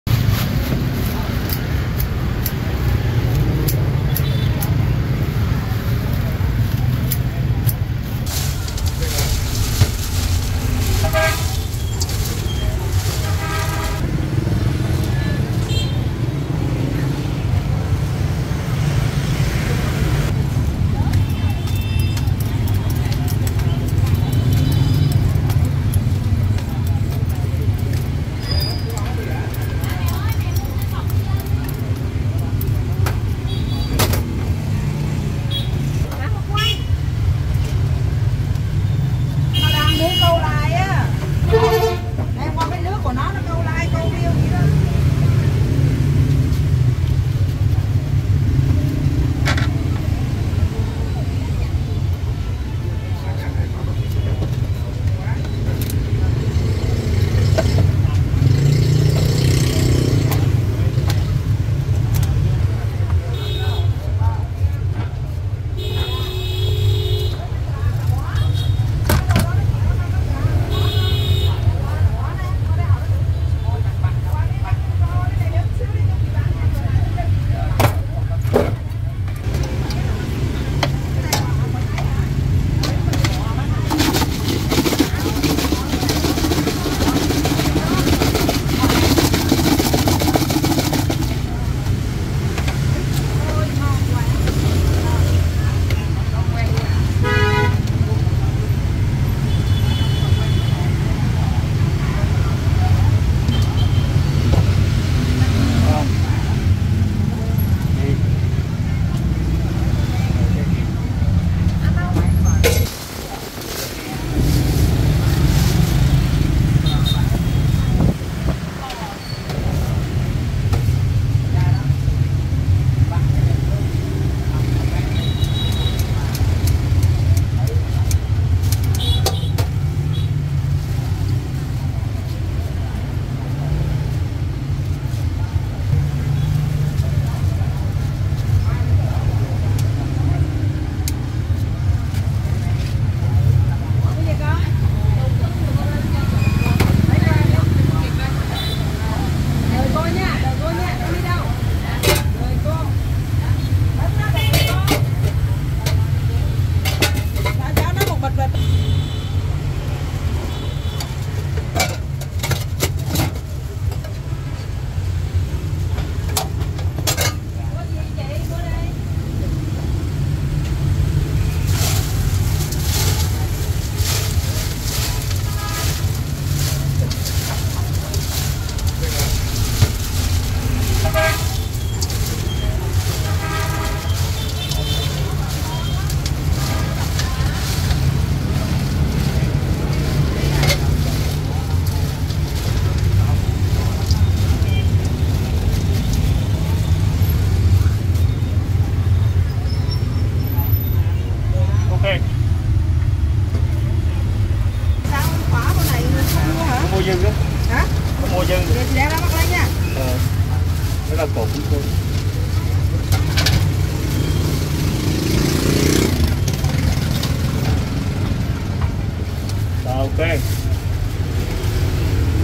Most Popular Mixed Rice Paper of Vietnam | Vietnamese Street Food | Amazing Spicy Mixed Rice Paper
MIXED RICE PAPER: Ho Chi Minh City, Vietnam (Vietnam). Daily Sale It's Very Tasty Street Food You Can Visit Eating This Tasty Food Price - 20k VND. Thank You.